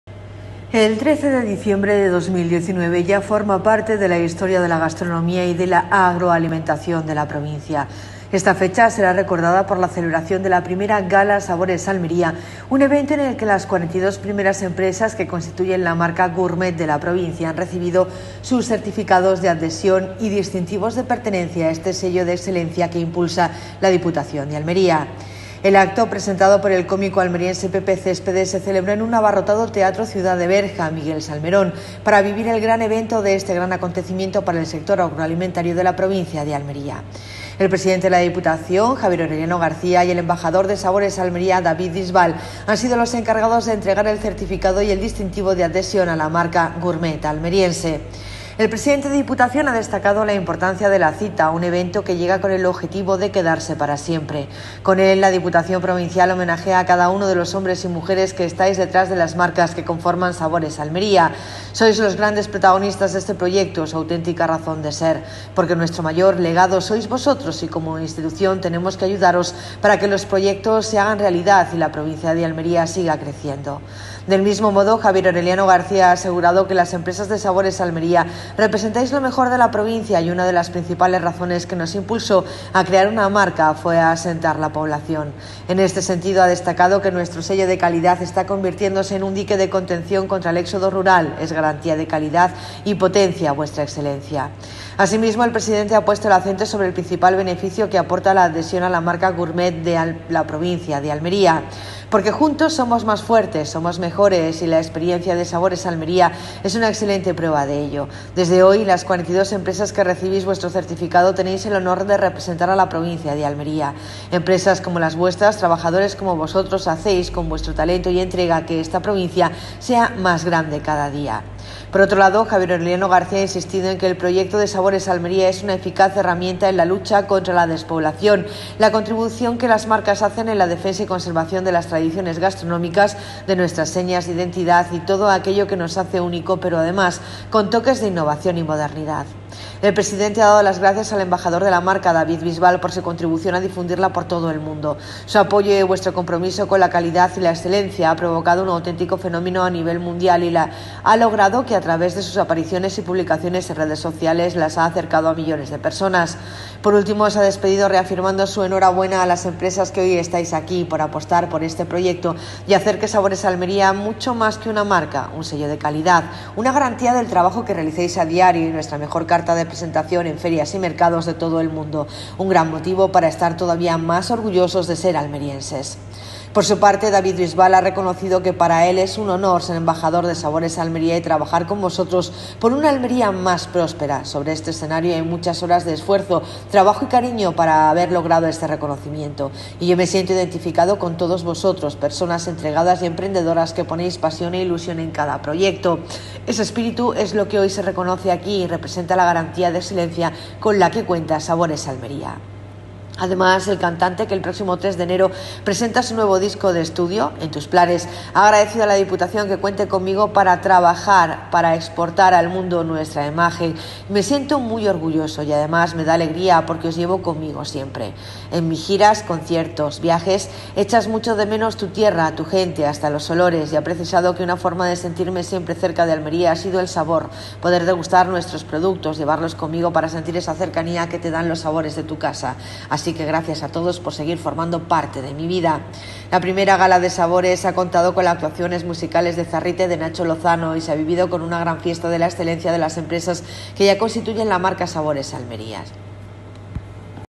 ‘Sabores Almería’ entrega sus distinciones con su embajador David Bisbal
‘Sabores Almería’ entrega sus distinciones a las primeras empresas de la marca con su embajador David Bisbal